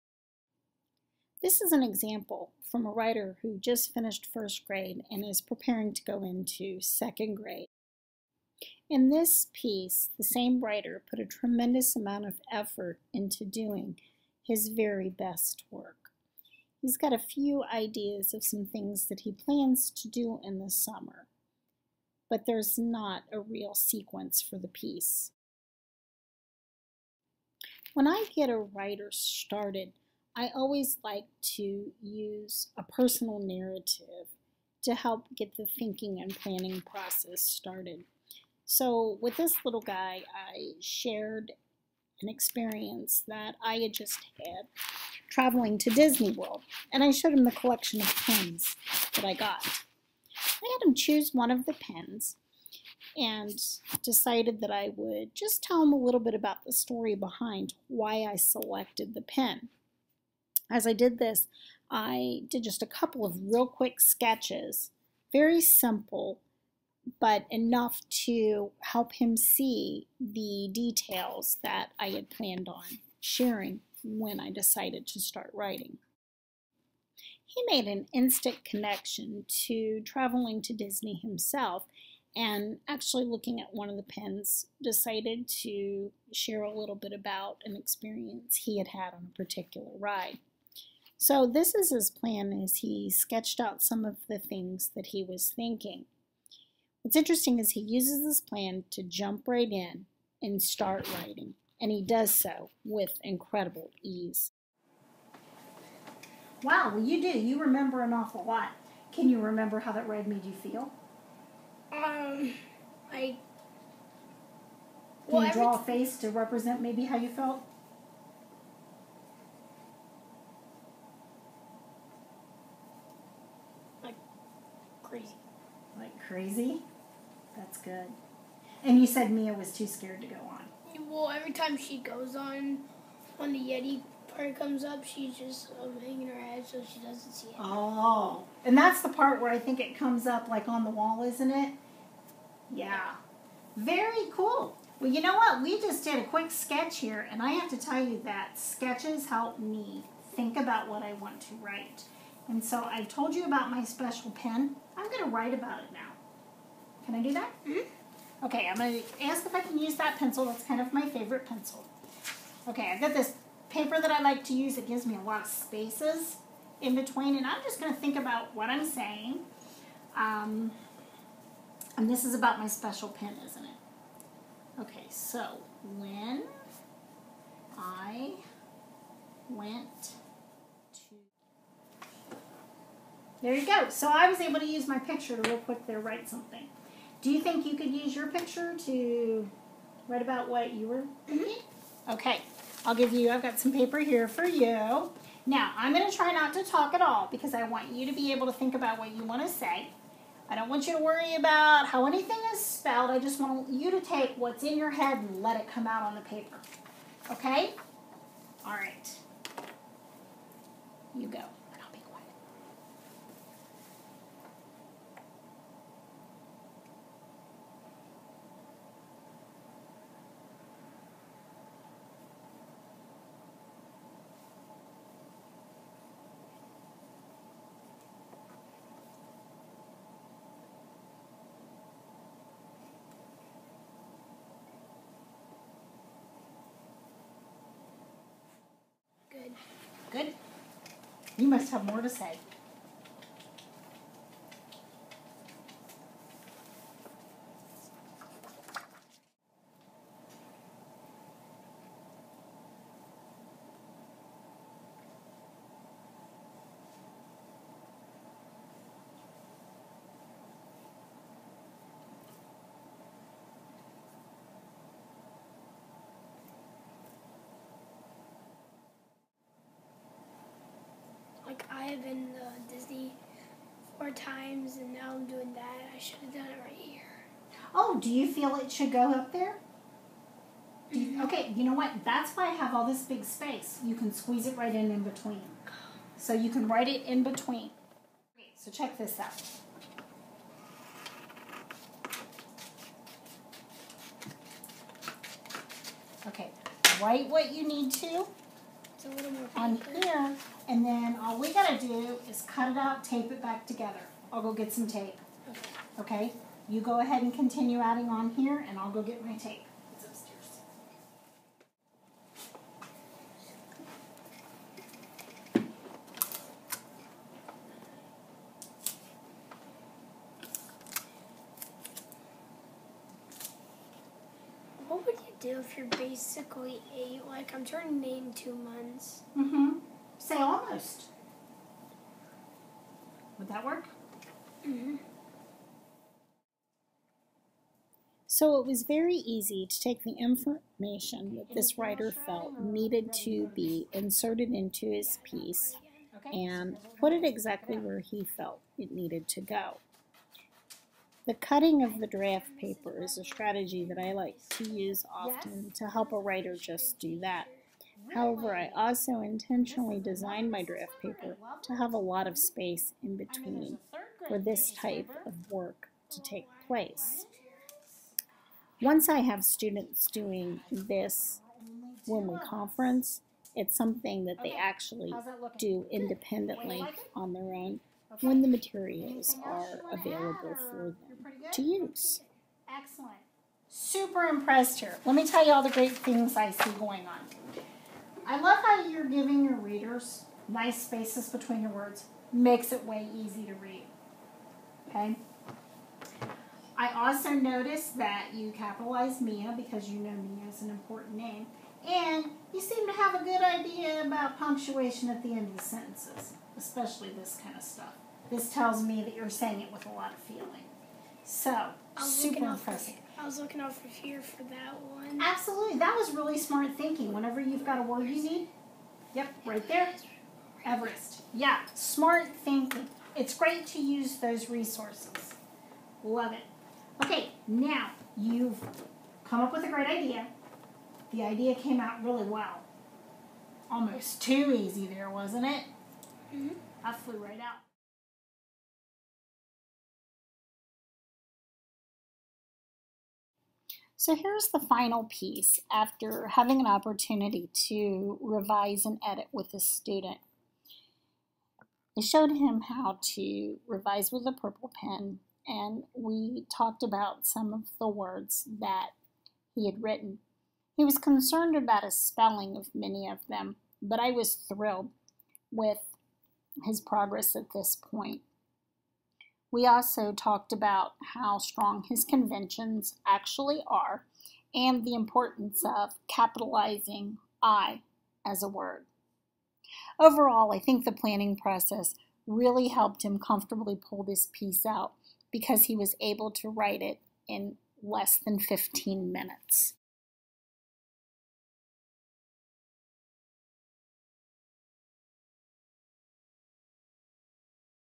Starting a writer with the planning process
Helping ideas flow when writing is the key.  When a student has a plan in mind, drafting becomes very easy.